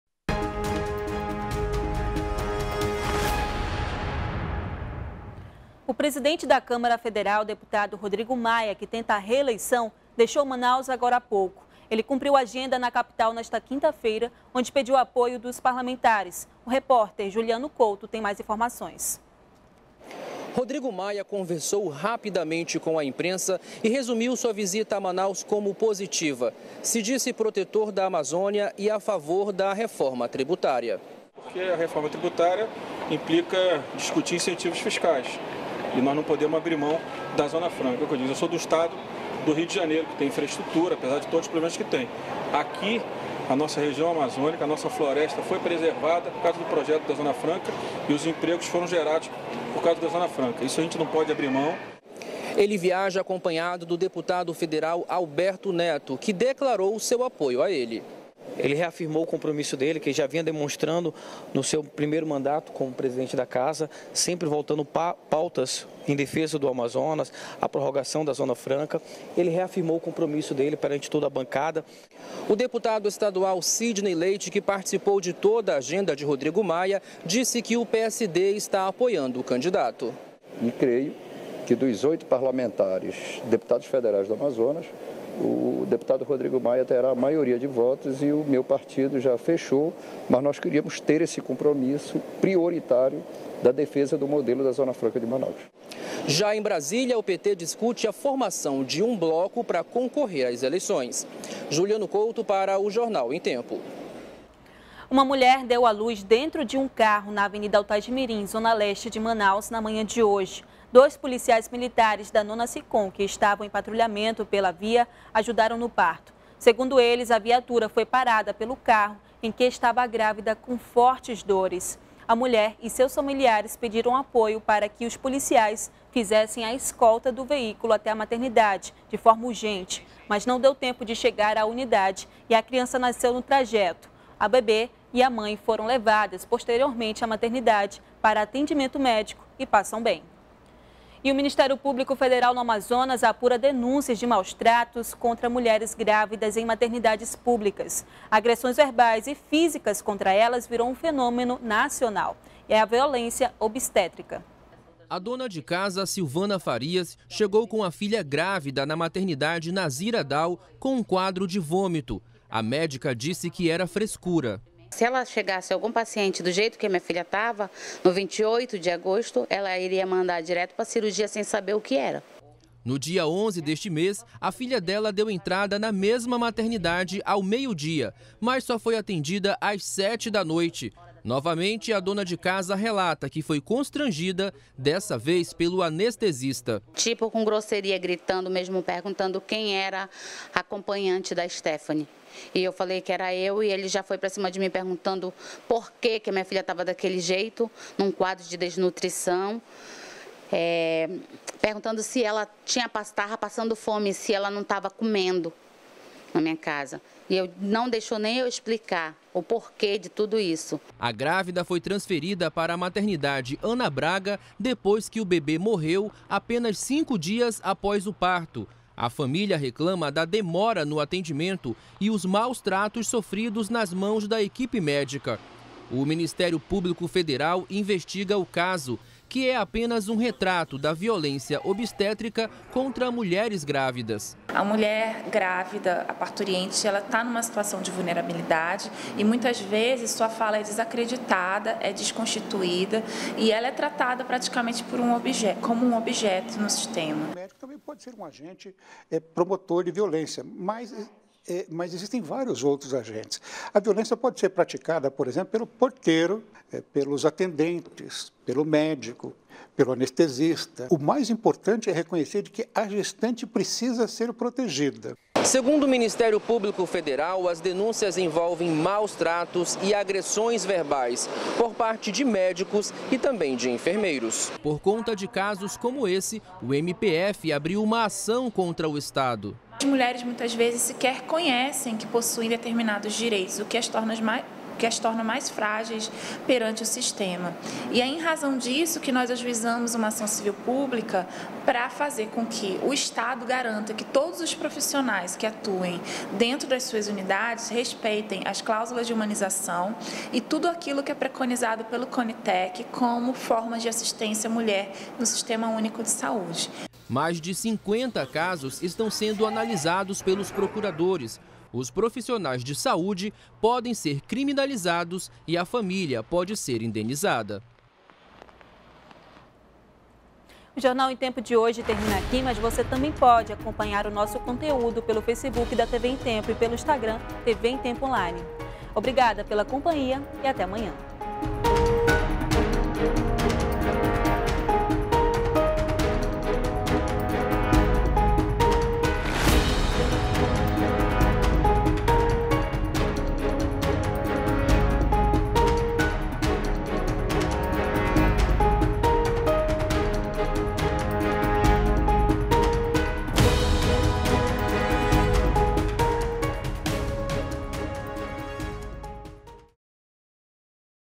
MPF no Amazonas apura denúncias de maus-tratos contra mulheres grávidas em maternidades públicas
03º BLOCO JORNAL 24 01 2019 - MPF no Amazonas apura denúncias de maus-tratos contra mulheres grávidas em maternidades públicas